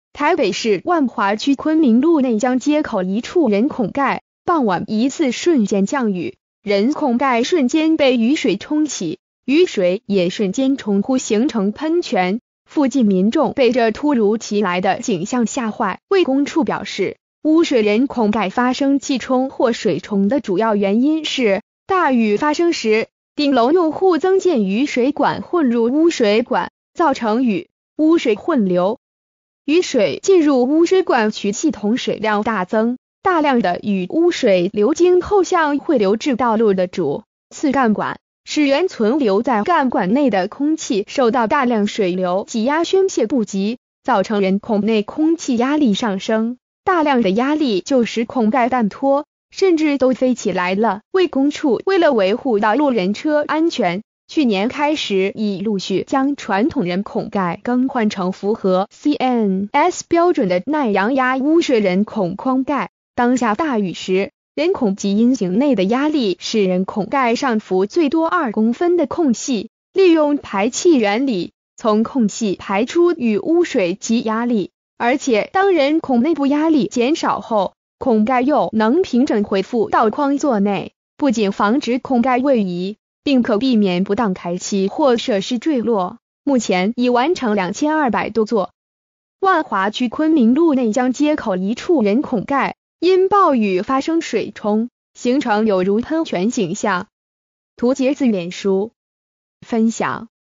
西門町人孔蓋出現「噴泉」 工務局：瞬間水量暴增衝開
發表評論評論如下。